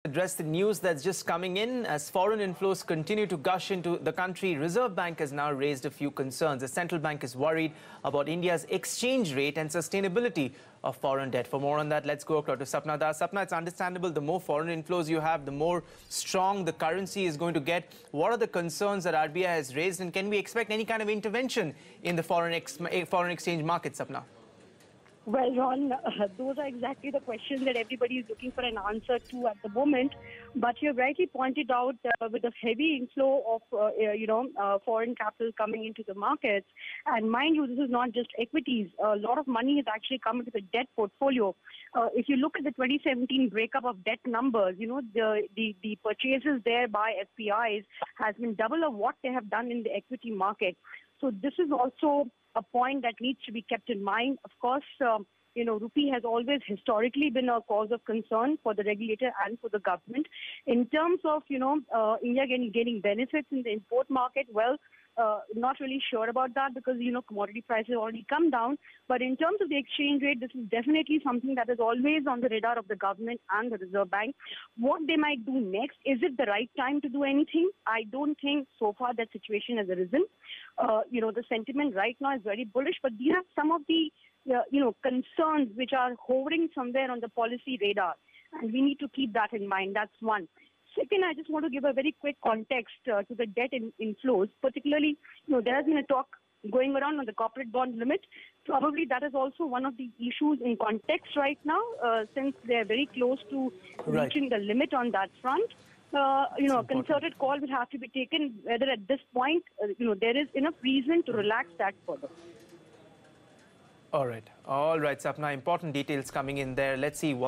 RBI Concerned By Rising Forex Inflows
As Foreign Inflows continue to gush into India, the Reserve Bank has a few concerns, the Central Bank is worried about India's exchange rate and sustainability of foreign debt. Sapna Das reports